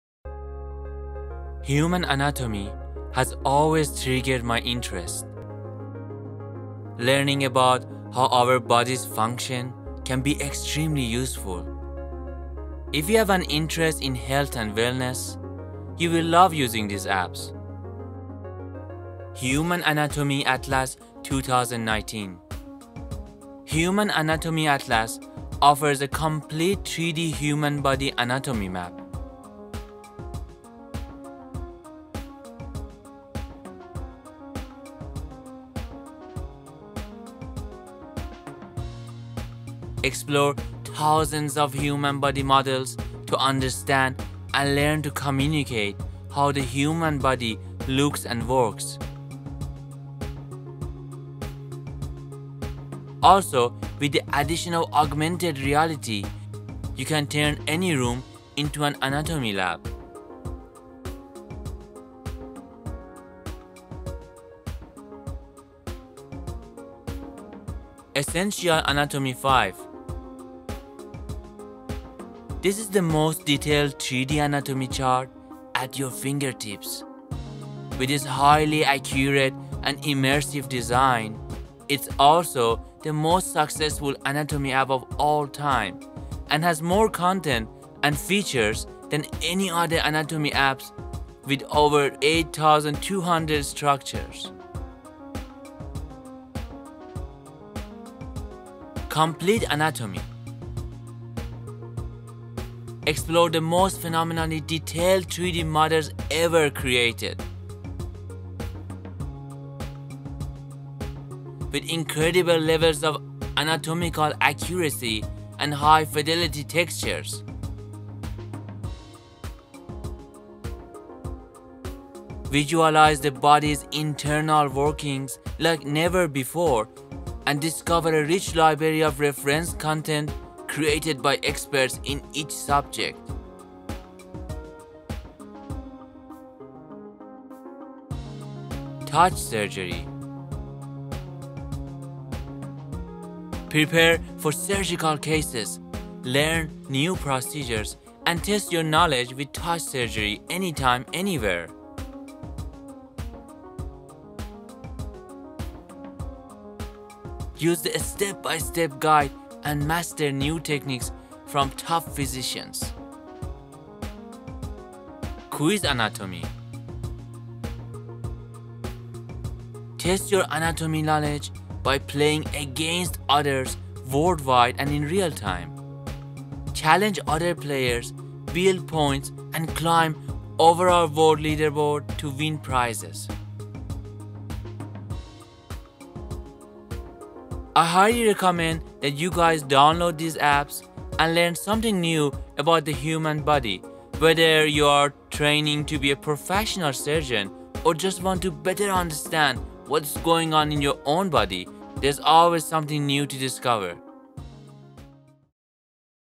Human Anatomy Apps for Iphone (Apps for Iphones/Ipads in the App Store)
Human anatomy has always triggered my interest. Learning about how our bodies function can be extremely useful. If you have an interest in health and wellness, you will love using these apps.

Human Anatomy Atlas 2019
Human Anatomy Atlas offers a Complete 3D Human Body anatomy map.
Explore thousands of human body models to understand and learn to communicate how the human body looks and works. Also, with the addition of augmented reality, you can turn any room into an anatomy lab.

Essential Anatomy 5
This is The most detailed 3D anatomy chart at your fingertips. With its highly accurate and immersive design, It is also the most successful anatomy app of all time and has more content and features than any other anatomy app with over 8,200 structures.

Complete Anatomy
Explore the most phenomenally detailed 3D models ever created, with incredible levels of anatomical accuracy and high fidelity textures.
Visualize the body's internal workings like never before and discover a rich Library of reference content created by experts in each subject.

Touch Surgery
Prepare for surgical cases, learn new procedures, and test your knowledge with Touch Surgery anytime, anywhere.
Use the step by step guide and Master new techniques from top physicians.

Quiz Anatomy
Test your anatomy knowledge by playing against others worldwide and in realtime.
challenge other players, build points and climb the overall World Leaderboard to win prizes.

I highly recommend that you guys download these Apps and learn something new about the human body. Whether you are training to be a professional surgeon, or just want to better understand what is going on in your own body, there is always something new to discover.